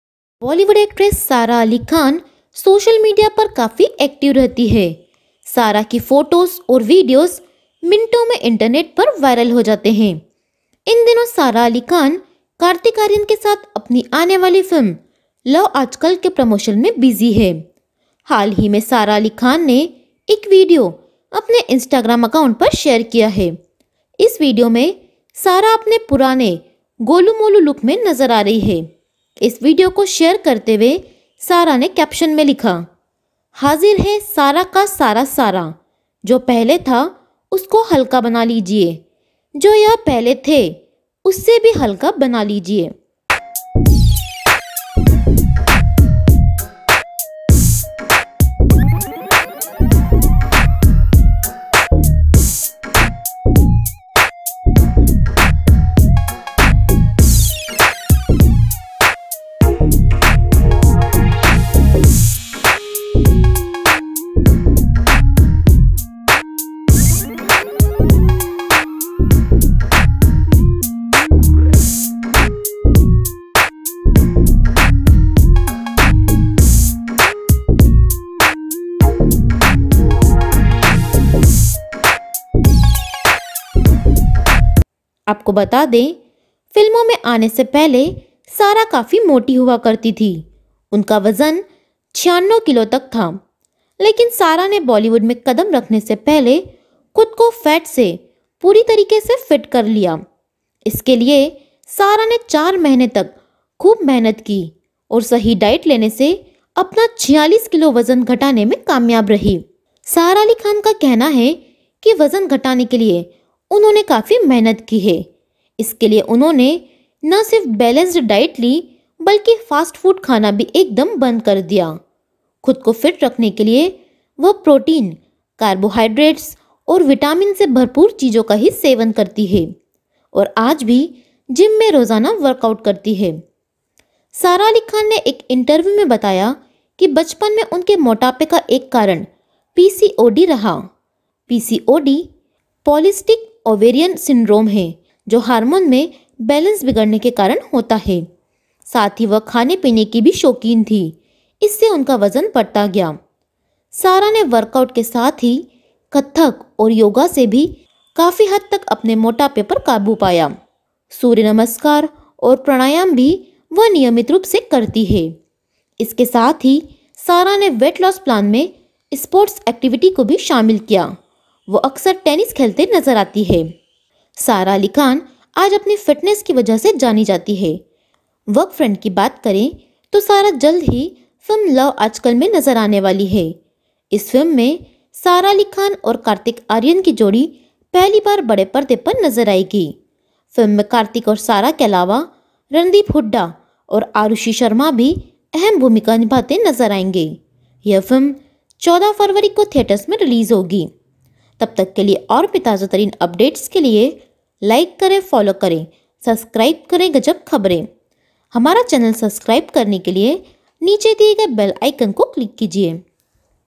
सारा ने शेयर किया खुद के मोटापे वाले दिनों का वीडियो, फिल्मों के लिए घटाया इतना वजन | Sara Ali Khan
Sara Ali Khan shares a throwback video before she got into acting and we can’t stop commending her massive transformation
सारा अली खान ने एक वीडियो अपने इंस्टाग्राम एकाउंट द्वारा शेयर किया है।  इस वीडियो में सारा अपने पुराने गोलू मोलू लुक में नजर आ रही है इस वीडियो को शेयर करते हुए सारा अली खान ने कैप्शन में लिखा, "हाजिर है सारा का सारा सारा।" "जो पहले था, उसको 'हल्का' बना लीजिए. जो यह पहले थे, उससे भी 'हल्का' बना लीजिए."